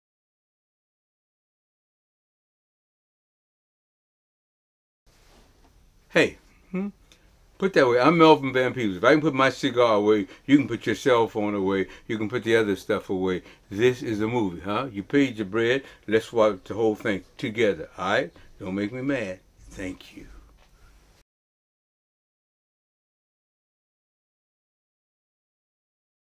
Melvin Van Peebles tells you to put away that cell phone
Recorded in Northwest Film Forum's 16mm film vault during Melvin's visit on September 9, 2009